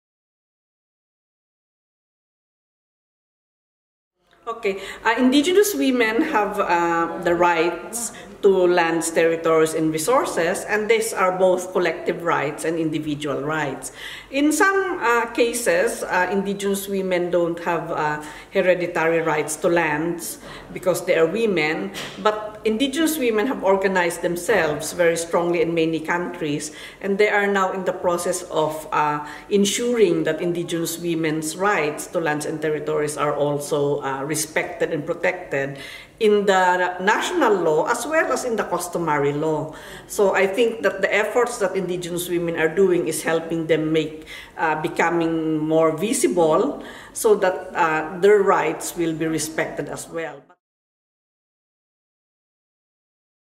Vicky Tauli-Corpuz-Indigenous Women Campaign 2018 (1)
Indigenous women are key allies in the fight against hunger and malnutrition. They contribute to sustainable livelihoods as food producers, guardians of native seeds and custodians of traditional knowledge. Yet they are invisible, and their rights are not recognized nor protected.

The Global Campaign for the Empowerment of Indigenous Women for Zero Hunger, launched by FAO, FIMI and Notimia, aims at making indigenous women’s challenges and contributions visible as a necessary step to “leave no one behind” in the achievement of the Sustainable Development Agenda.